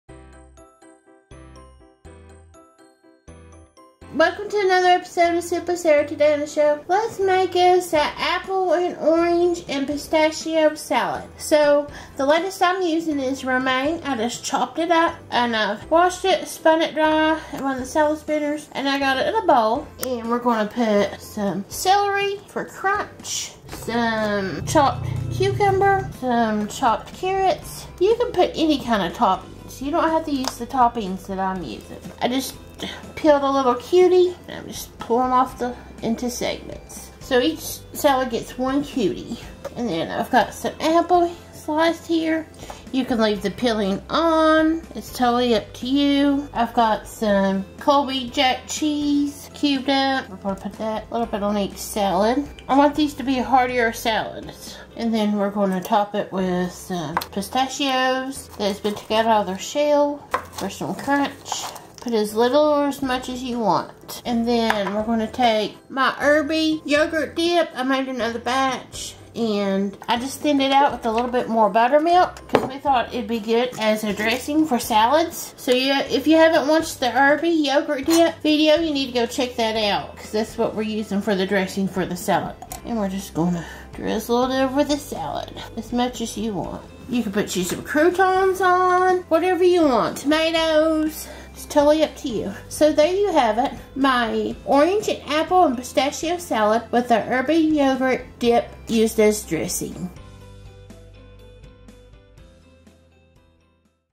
Apple Orange & Pistachio Salad
For the dressing I used my herby yogurt dip recipe and thinned it out with buttermilk to make a dressing for this salad.